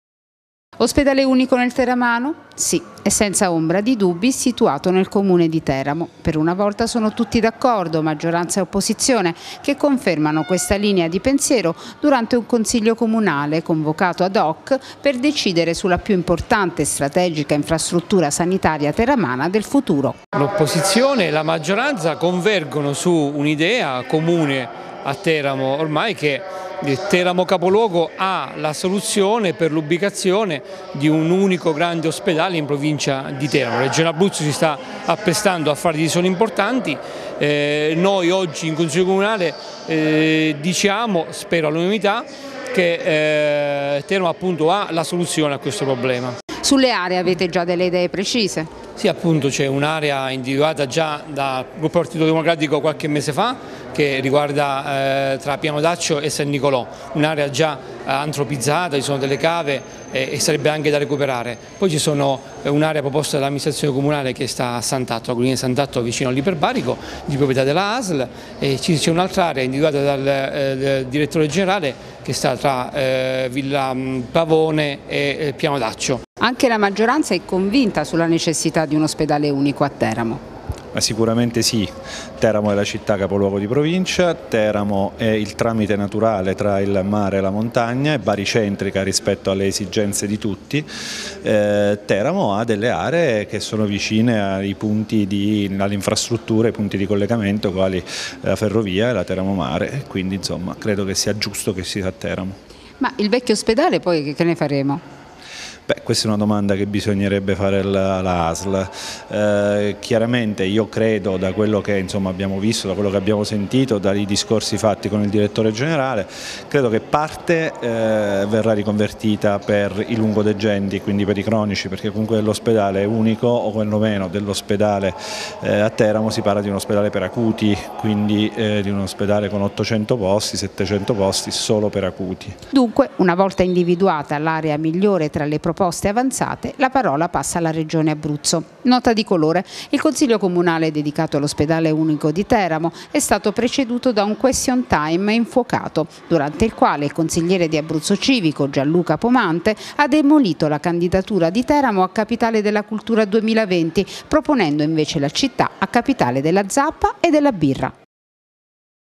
Ospedale unico: tutti d'accordo
Idea unanimemente condivisa da opposizione e maggioranza sulla necessità di un ospedale unico a Teramo capoluogo. La futura infrastruttura sanitaria sarà di 800 posti destinati agli acuti e sorgerà tra San Nicolò e Teramo città.